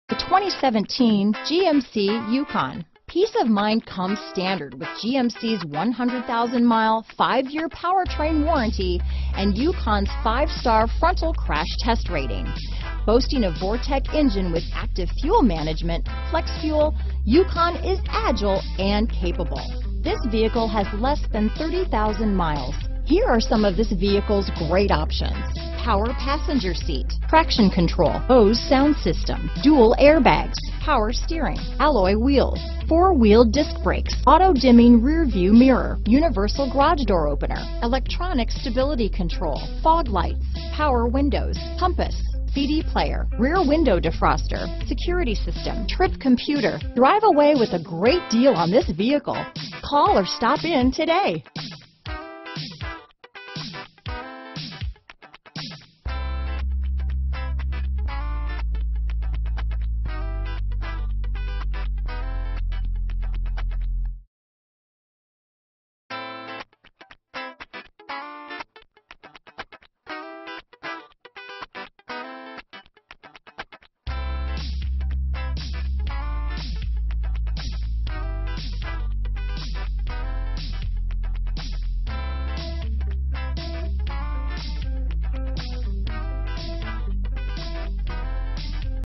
2017 GMC Yukon Colonie, Albany, Saratoga Springs, Clifton Park, Schenectady, NY PR2117
DeNooyer Chevrolet
127 Wolf Road

4WD. Gasoline! Hurry and take advantage now! How would your family like to hear that you are driving home in this charming 2017 GMC Yukon? There aren't any used vehicles more reliable than a GMC, unless it's a GMC with low mileage like this 2017 Yukon.